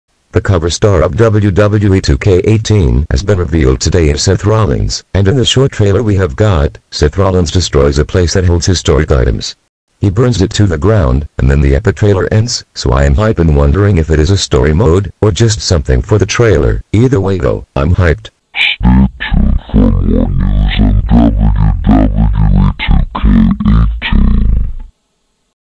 WWE 2K18 COVER STAR REVEALED!!!!! SETH ROLLINS!!!!!!!
THE WWE COVER STAR HAS BEEN REVEALED AS SETH FREAKIN ROLLINS EARLIER ON ESPN SPORTSCENTER